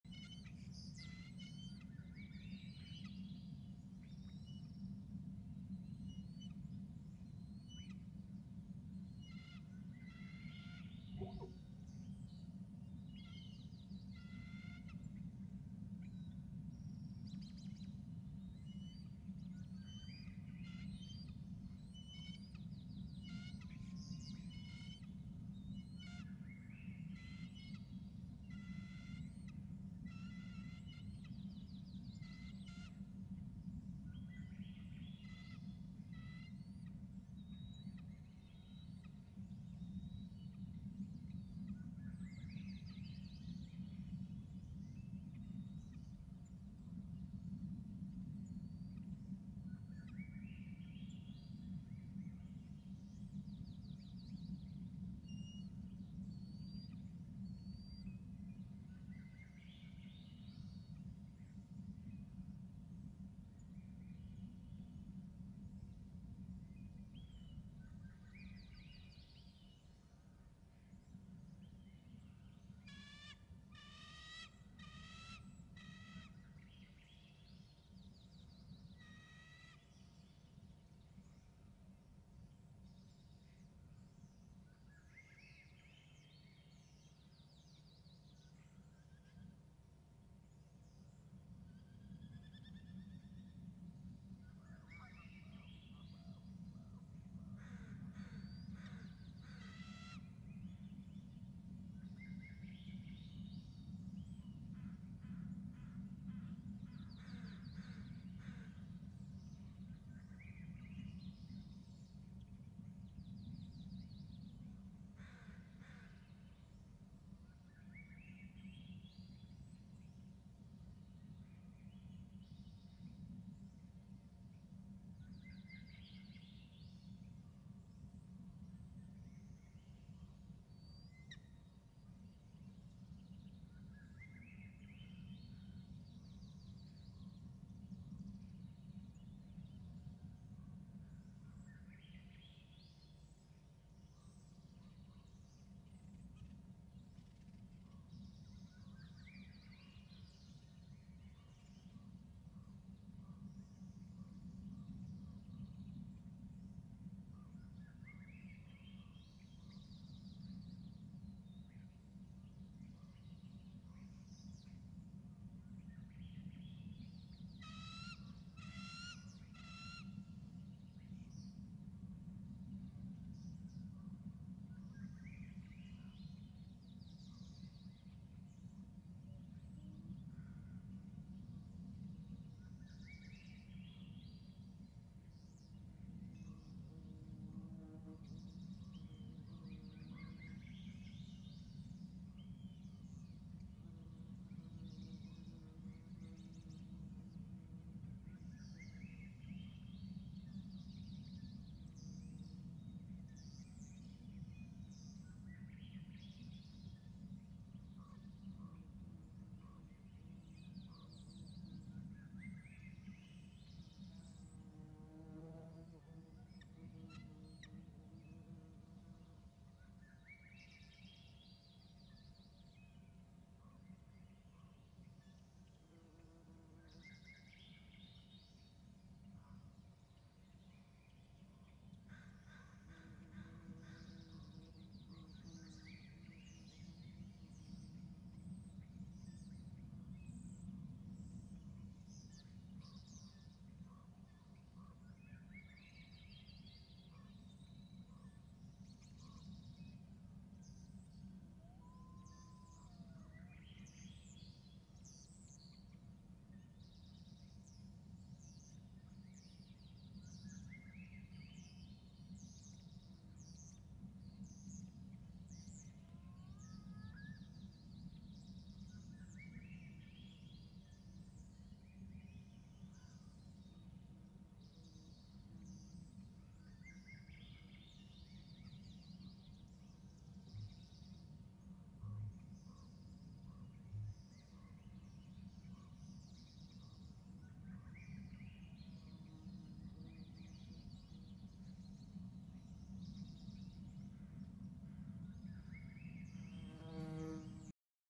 6.22 Mr on nest, eaglet poops on Mr
6.22 Mr on nest, eaglet poops on Mr